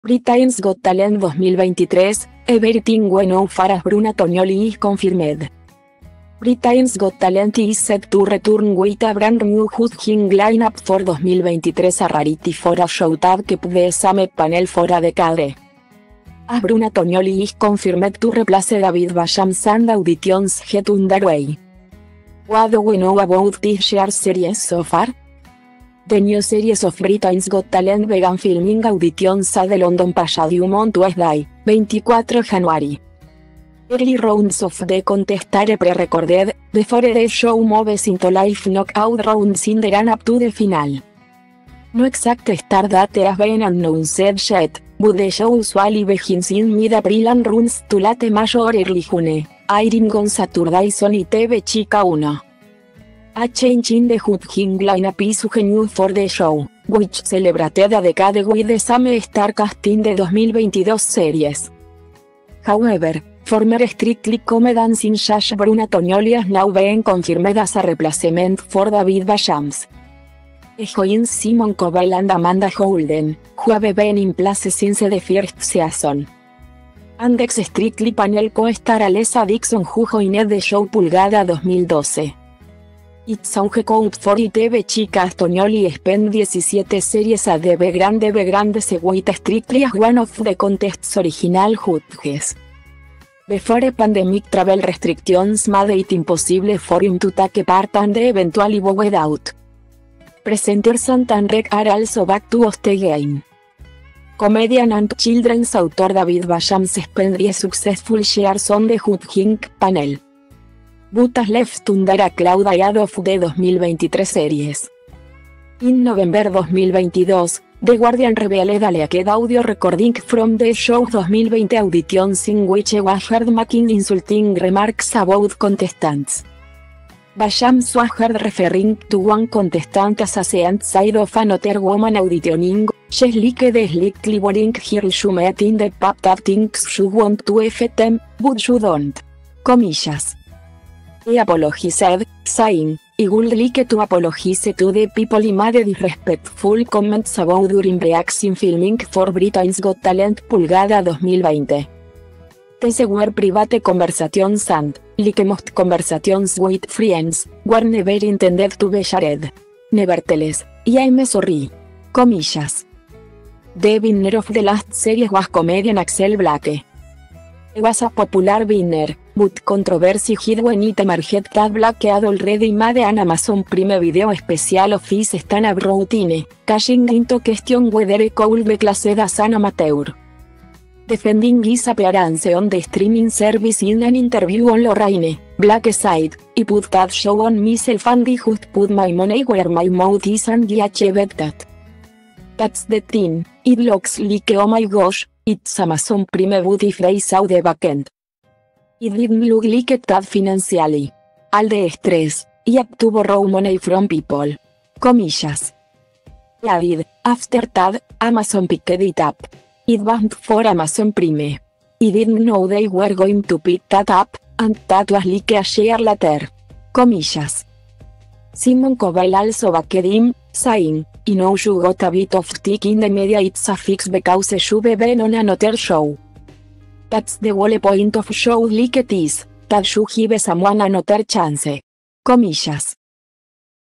Britain's Got Talent 2023: Everything we know far as Bruno Tonioli is confirmed
Britain's Got Talent is set to return with a brand new judging line-up for 2023 - a rarity for a show that kept the same panel for a decade. As Bruno Tonioli is confirmed to replace David Walliams and auditions get under way,what do we know about this year's series so far?